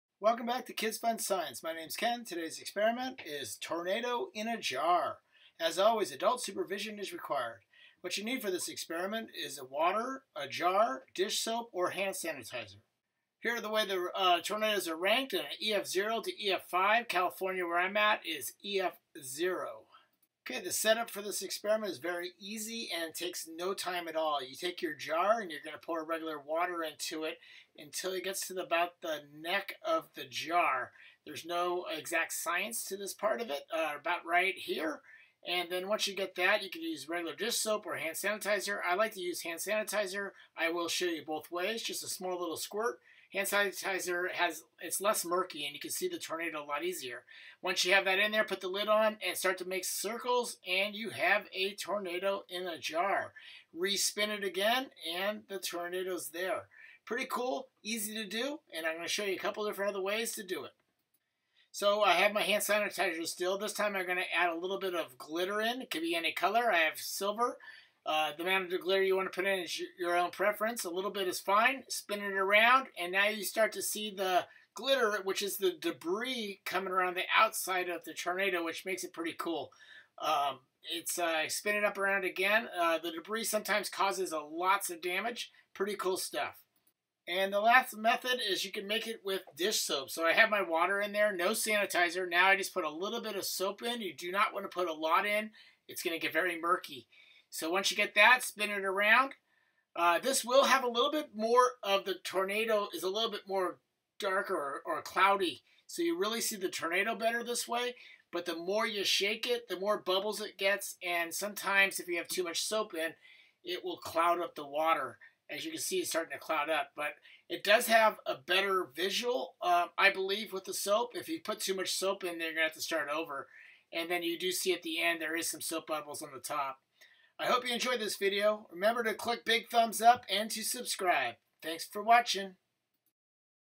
DIY Tornado in a Jar | Fun Science Experiments for Kids
TORNADO in a jar experiment

Plastic Jars

Chapters
0:07 What you need
0:20 or 1:27 Setup for this experiment
0:47 (hand sanitizer) & 1:10 (W/glitter) & 1:37 (soap)The experiment
1:03 Adding extra to the experiment

Science Behind it
When you spin the water in the jar, it creates a vortex in the center. As the water spins, centripetal force causes the water to spin around that vortex making a mini tornado. {Hurricanes have a vortex too.}

Remember to just add a small amount of dish soap or it might get to cloudy.

The swirling motion you give the jar forms a vortex and is a easy way to create your own tornado. You can add food coloring or glitter to add some awesome tornados.

Tornado alley is region in the Central United States where tornados often occur. The states of Texas, Oklahoma, Nebraska, Kansas, Iowa and Missouri are in Tornado Alley.

The Fujita Scale

In 1971 Dr T. Theodore Fujita developed a scale to classify a tornado's speed and potential for damage. Tornados are rated on the Fujita scale after they have passed through an area and damage has been assessed.

Made for parents and teachers! 🧑‍🏫 Learn how to make a captivating "tornado in a bottle" with this simple "homemade science" experiment. We explore the fascinating "vortex" phenomenon, demonstrating "how to make tornado" effects using just water and soap, creating an amazing "water tornado" right in your jar. This educational video provides an engaging activity for all ages.